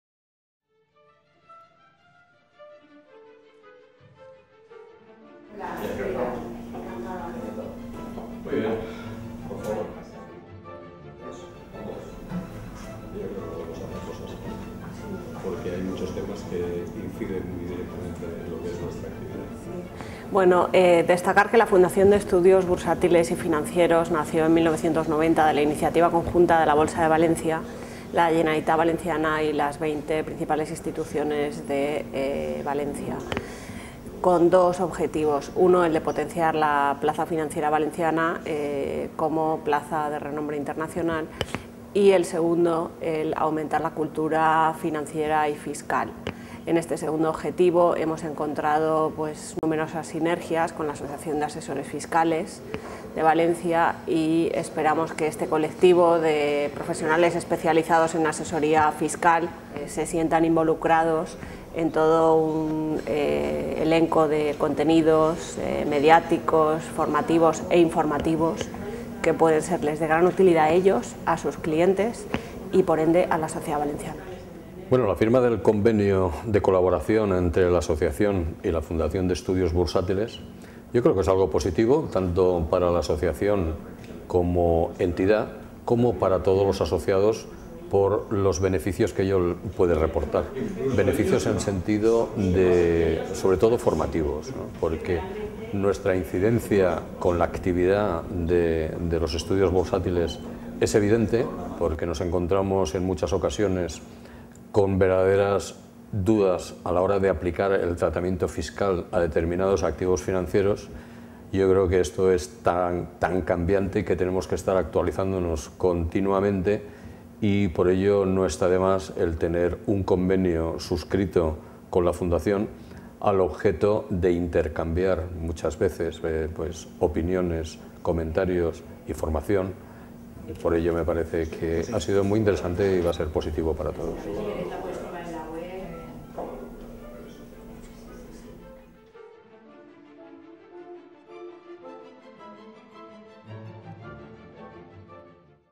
APAFCV convenio FEBF
El pasado martes, 5 de julio de 2011, coincidiendo con la celebración de los 26 años de existencia de la Asociación Profesional de Asesores Fiscales de la Comunidad Valenciana, y contando con la presencia de la Directora Gral. de FEBF, Isabel Giménez, de su directora financiera, Leonor Vargas, del presidente de Apafcv, Arturo Casinos, y del secretario de nuestra entidad, Antonio Quereda, se firmó en nuestra sede de Valencia el acuerdo de colaboración con la Fundación de Estudios Bursátiles y Financieros.

Por medio de este convenio ambas partes se comprometen a colaborar y participar en acciones de formación promovidas por FEBF y APAFCV, siempre que resulten de interés para ambos colectivos. En este sentido, ambas entidades podrán beneficiarse de los descuentos en las actividades formativas y la FEBF se presta a facilitar a la Asociación aquéllos ponentes que precise sobre el tema financiero.